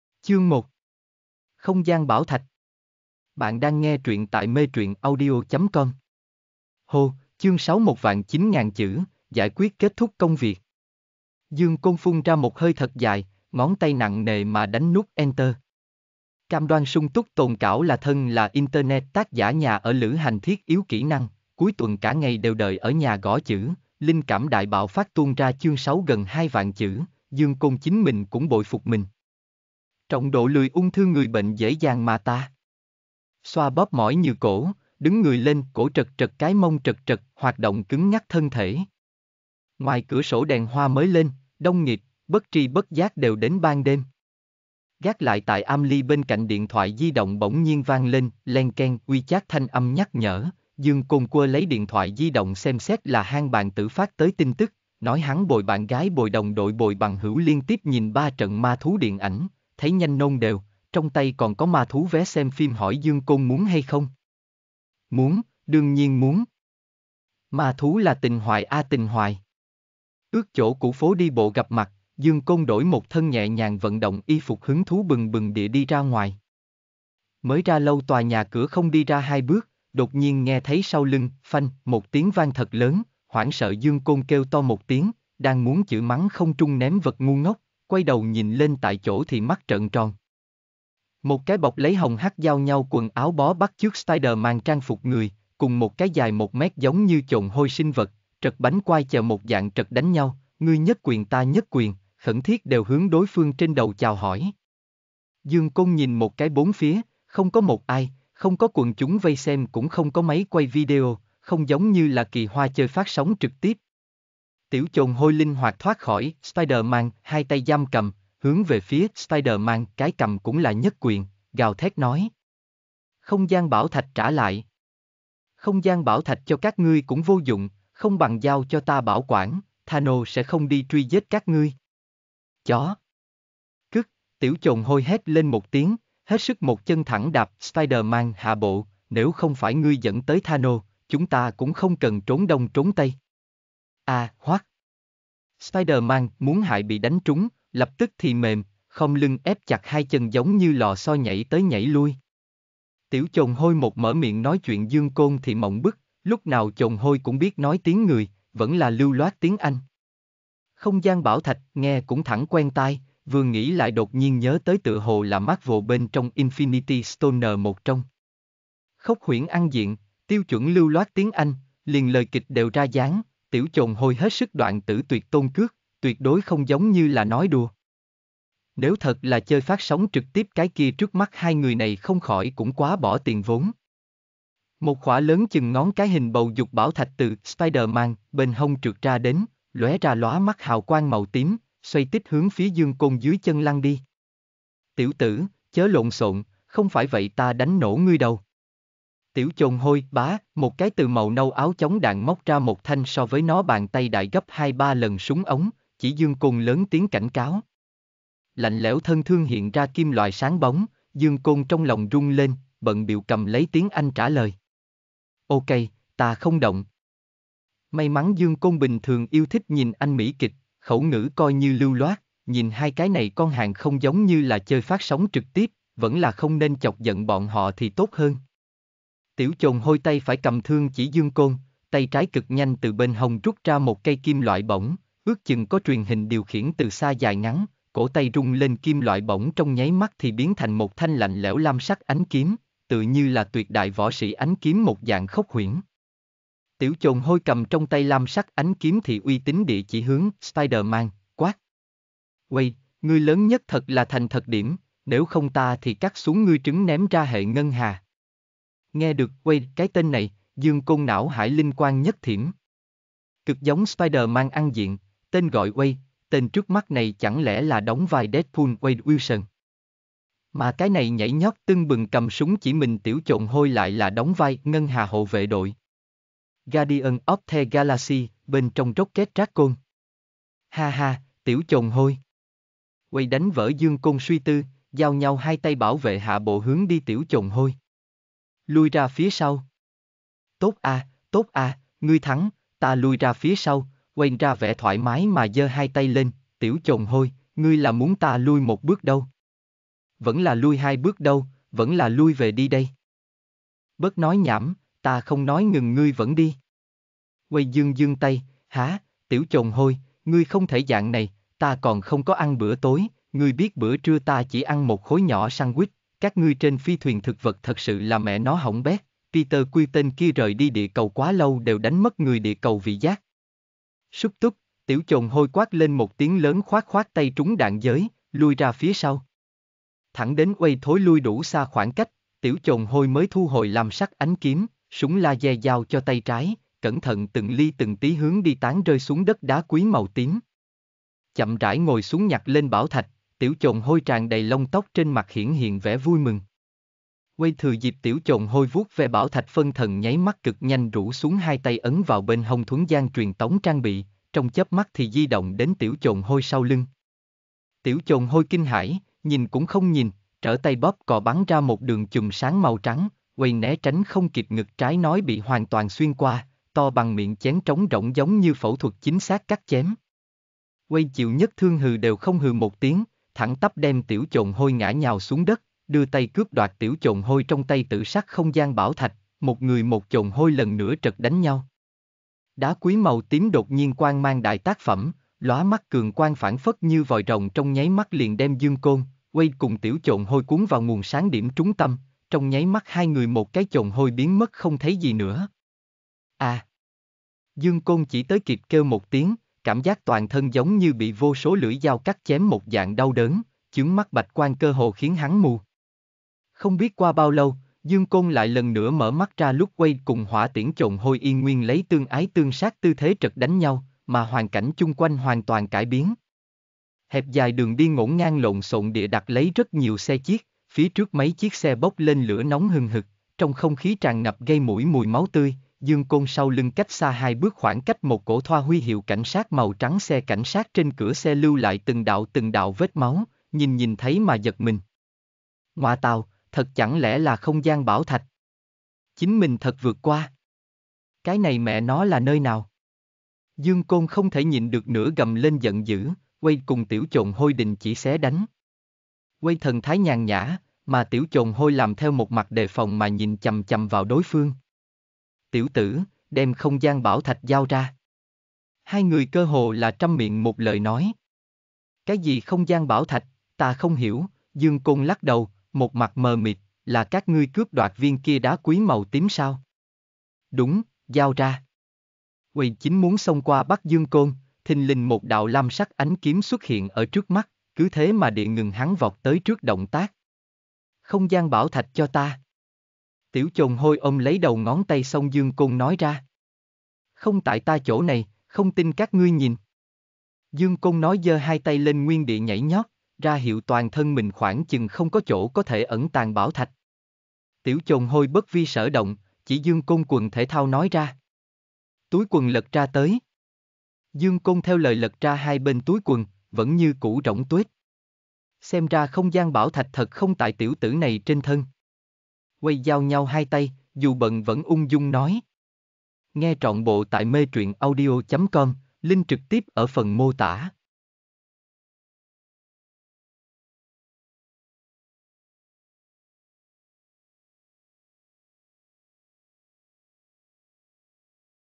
Marvel Vô Song Audio Trọn Bộ
Truyện: MARVEL VÔ SONG Tập 1
Tác giả: Tây Môn Khánh Chi
Thể loại: Khoa Huyễn